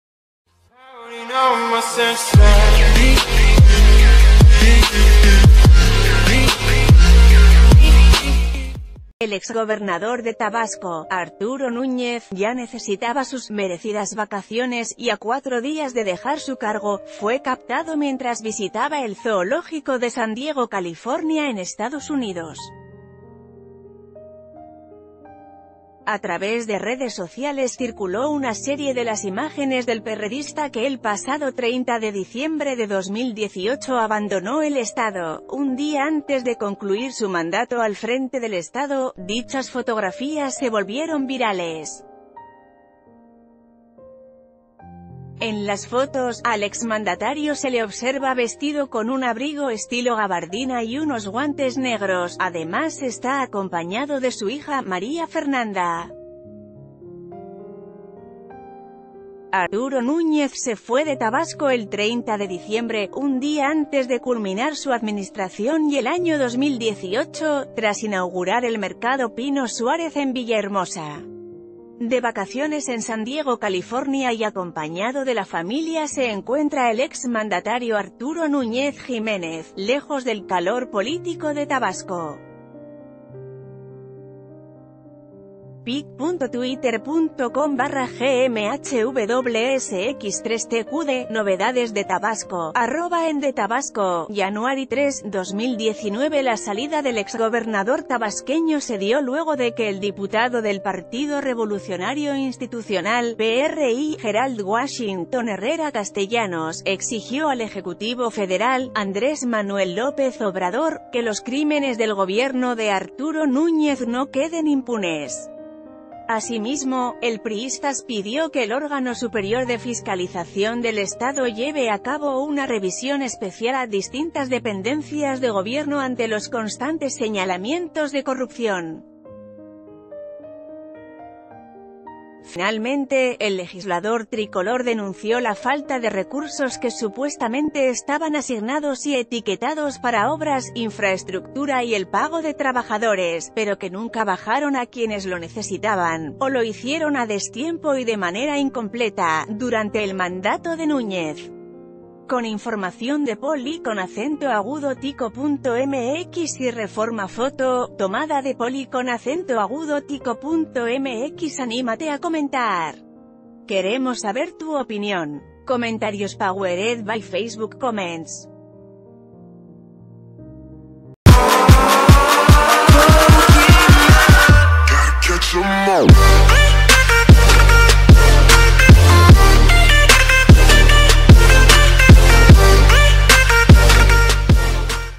Pese a señalamientos por endeudamiento, exgóber de Tabasco se pasea en San Diego
El exgobernador de Tabasco, Arturo Núñez, ya necesitaba sus ‘merecidas’ vacaciones, y a cuatro días de dejar su cargo, fue captado mientras visitaba el zoológico de San Diego, California en Estados Unidos.